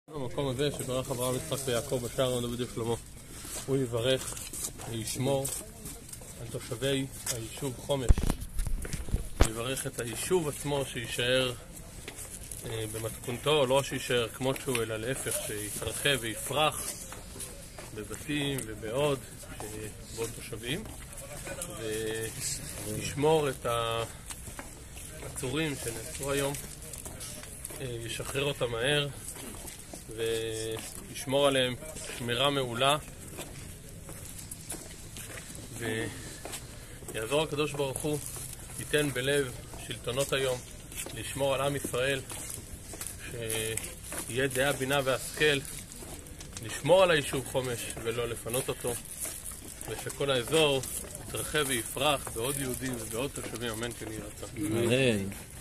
תפילה בהר הבית על חומש ועל חלוציה
תפילה על עצורי חומש • שמור נא על תושבי חומש • שמור על הישוב חומש שיגדל ויפרח • הרב אליהו ובר בהר הבית